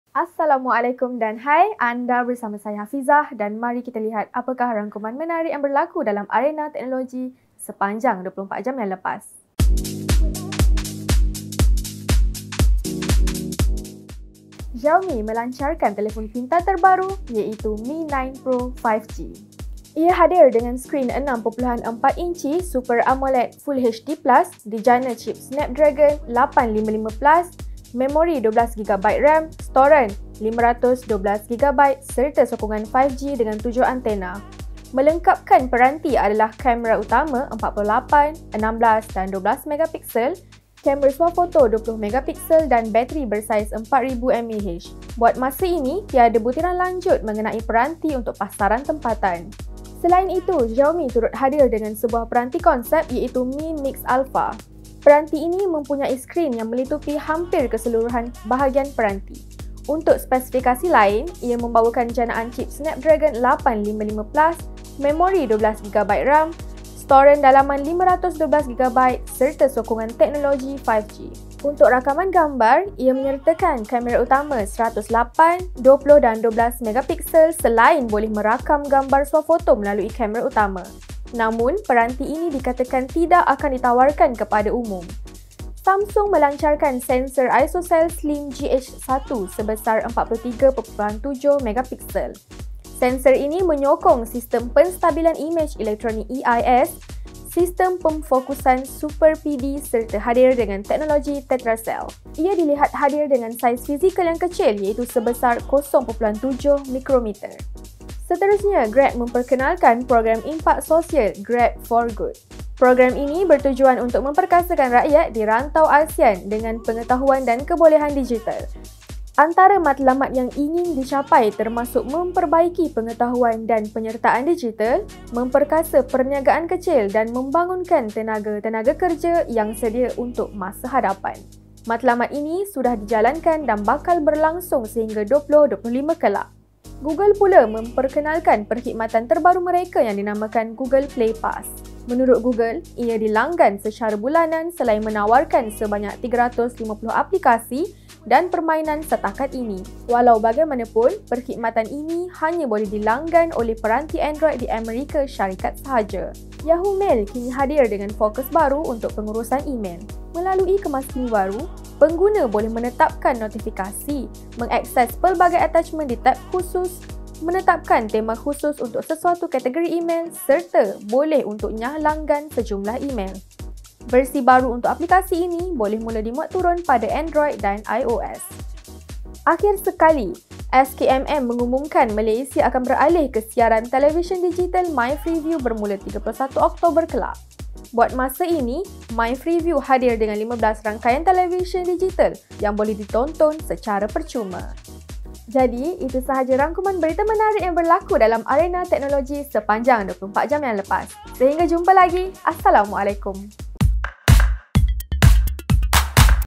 Amanz24 - Mi Mix Alpha, Mi 9 Pro 5G, Google Play Pass
Xiaomi telah melancarkan telefon pintar terbaru, iaitu Mi 9 Pro 5G. Ia hadir dengan skrin 6.4-inci Super AMOLED FHD+, dijana cip Snapdragon 855+, memori 12GB RAM, storan 512GB serta sokongan 5G dengan 7 antena. Melengkapkan peranti adalah kamera utama 48+16+12-megapixel, kamera swafoto 20-megapixel dan bateri bersaiz 4000mAh. Buat masa ini, tiada butiran lanjut mengenai peranti untuk pasaran tempatan.

Selain itu, Xiaomi turut hadir dengan sebuah peranti konsep iaitu Mi MIX Alpha. Peranti ini mempunyai skrin yang melitupi hampir keseluruhan bahagian peranti. Untuk spesifikasi lain, ia membawakan janaan cip Snapdragon 855+, memori 12GB RAM, storan dalaman 512GB serta sokongan teknologi 5G. Untuk rakaman gambar, ia menyertakan utama 108+20+12-megapixel selain boleh merakam gambar swafoto melalui kamera utama. Namun, peranti ini dikatakan tidak akan ditawarkan kepada umum.

Samsung melancarkan sensor ISOCELL Slim GH1 sebesar 43.7-megapixel. Sensor ini menyokong sistem penstabilan imej elektronik EIS, sistem pemfokusan Super PD serta hadir dengan teknologi Tetracell. Ia dilihat hadir dengan saiz fizikal yang kecil iaitu sebesar 0.7 mikrometer.

Seterusnya, Grab memperkenalkan program impak sosial Grab For Good. Program ini bertujuan untuk memperkasakan rakyat di rantau ASEAN dengan pengetahuan dan kebolehan digital. Antara matlamat yang ingin dicapai termasuk memperbaiki pengetahuan dan penyertaan digital, memperkasa peniagaan kecil dan membangunkan tenaga tenaga kerja yang sedia untuk masa hadapan. Matlamat ini sudah dijalankan dan bakal berlangsung sehingga 2025 kelak.

Google pula memperkenalkan perkhidmatan terbaru mereka yang dinamakan Google Play Pass. Menurut Google, ia dilanggan secara bulanan selain menawarkan sebanyak 350 aplikasi dan permainan setakat ini. Walau bagaimanapun, perkhidmatan ini hanya boleh dilanggan oleh peranti android di Amerika Syarikat sahaja.

Yahoo Mail kini hadir dengan fokus baru untuk pengurusan emel. Melalui kemaskini baru, pengguna boleh menetapkan notifikasi, mengakses pelbagai attachment di tab khusus, menetapkan tema khusus untuk sesuatu kategori emel serta boleh untuk nyah-langgan sejumlah emel. Versi baru untuk aplikasi ini boleh mula dimuat-turun pada Android dan iOS.

Akhir sekali, SKMM mengumumkan Malaysia akan beralih ke siaran televisyen digital MyFreeView bermula 31 Oktober kelak. Buat masa ini, MyFreeView hadir dengan 15 rangkaian televisyen digital yang boleh ditonton secara percuma.